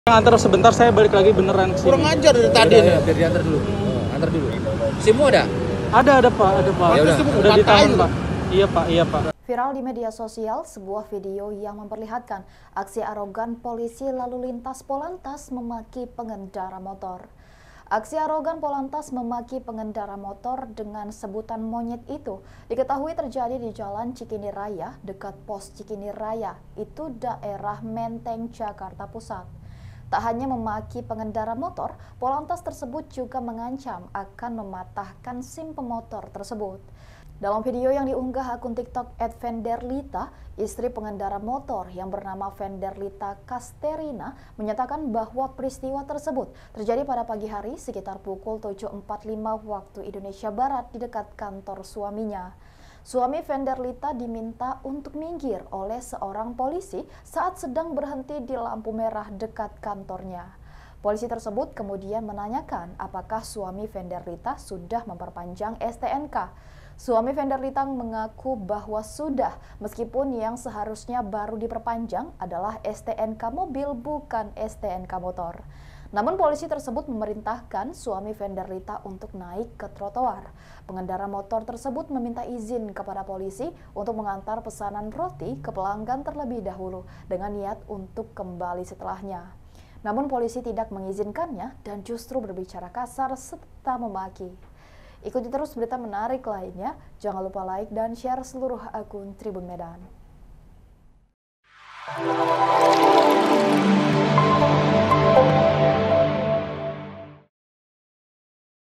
VIRAL!, POLANTAS Arogan Maki Pemotor 'Monyet' Setelah Terobos Lampu Merah, Ancam Patahkan SIM
Aksi arogan Polantas memaki pengendara motor dengan sebutan monyet itu diketahui terjadi di Jalan Cikini Raya, dekat pos Cikini Raya, itu daerah Menteng Jakarta Pusar.

Tak hanya memaki pengendara motor, Polantas tersebut juga mengancam akan mematahkan SIM pemotor tersebut.

Dalam video yang diunggah akun TikTok @fenderlita, istri pengendara motor yang bernama Fenderlita Kasterina menyatakan bahwa peristiwa tersebut terjadi pada pagi hari sekitar pukul 07:45 WIB di dekat kantor suaminya.